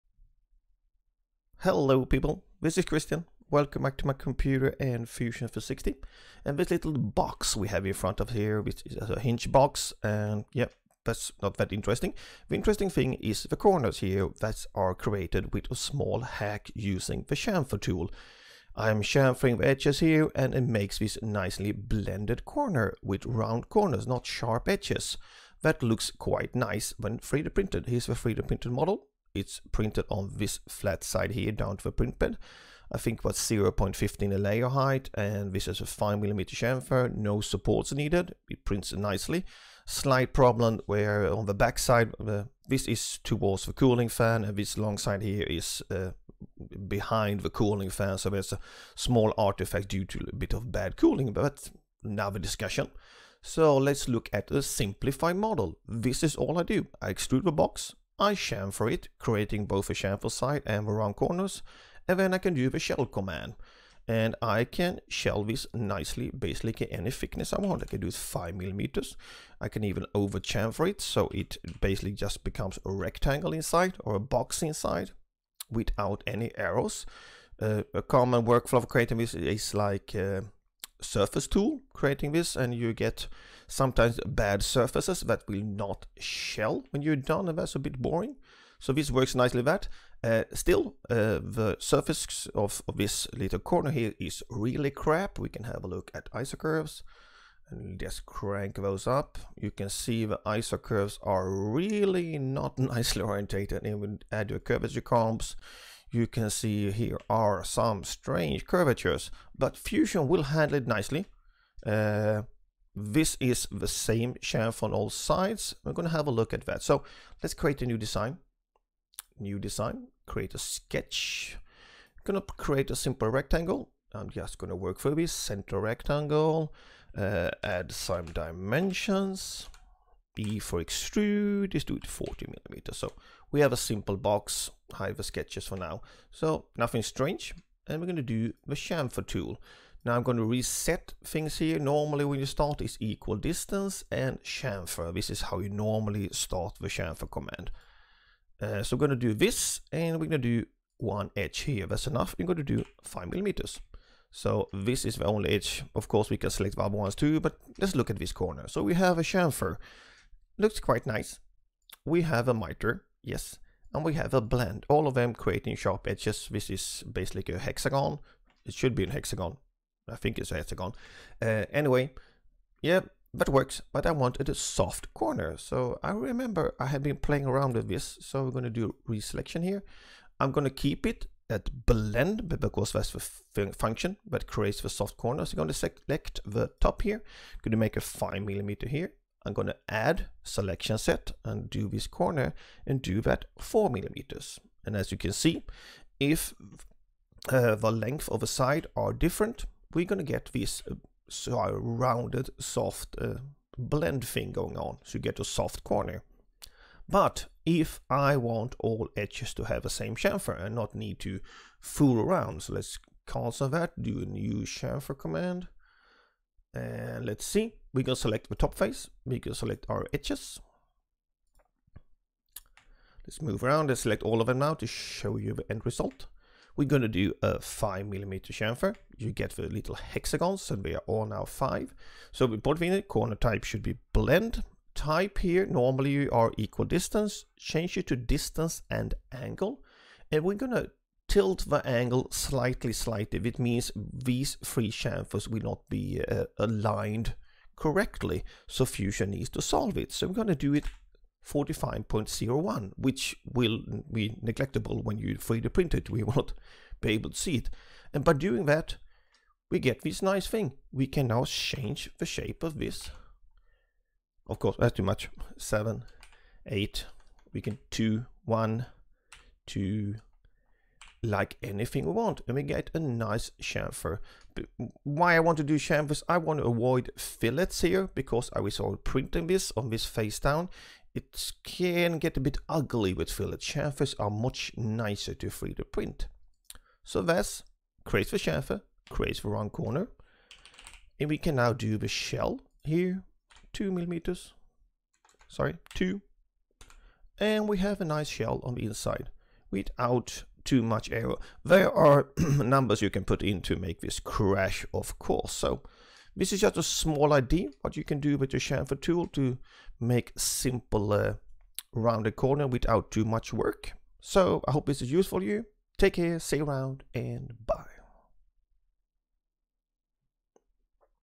Fusion 360 - Make a Box with round corners using a Chamfer trick
Here is a workflow on how to easily create rounded corners using the Chamfer tool

0:00 Intro
0:26 3D printed box
0:55 More info and surface curvature
2:00 The Workflow